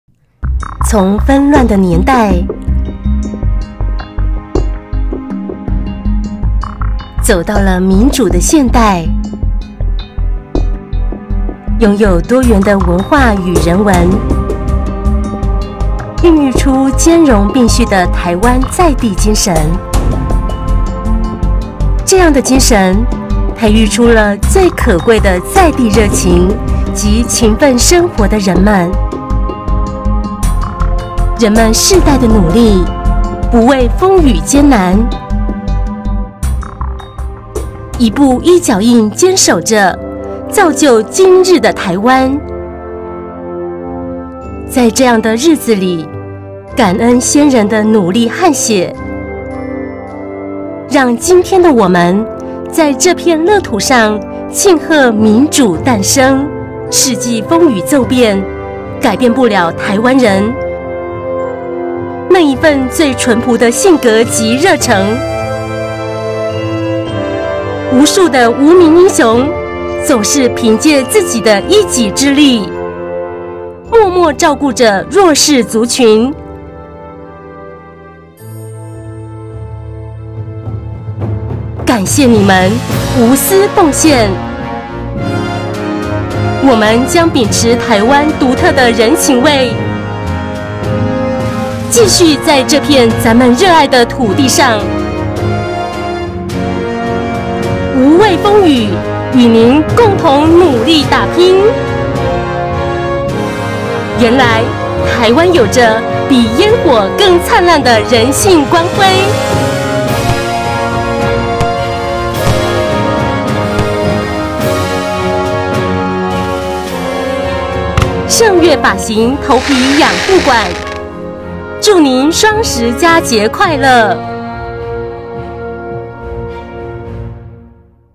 勝悅髮型頭皮養護館祝您雙十佳節快樂
從紛亂的年代走到了民主的現代
擁有多元的文化與人文
孕育出兼容並蓄的台灣在地精神
這樣的精神培育出了
最可貴的在地熱情及勤奮生活的人們

人們世代的努力，不畏風雨艱難
一步一腳印堅守著，造就今日的臺灣
在這樣的日子裡，感恩先人的努力汗血
讓今天的我們，在這片樂土上慶賀民主誕生

世紀風雨驟變改變不了臺灣人
那一份最淳樸的性格及熱誠
無數的無名英雄，總是憑藉自己的一己之力
默默照顧著弱勢族群
感謝您們、無私奉獻

我們將秉持台灣獨特的人情味
繼續在這片咱們熱愛的土地上
無畏風雨與著您共同的努力打拼

原來台灣有著比煙火更燦爛的人性光輝

雙十佳節快樂

勝悅髮型頭皮養護館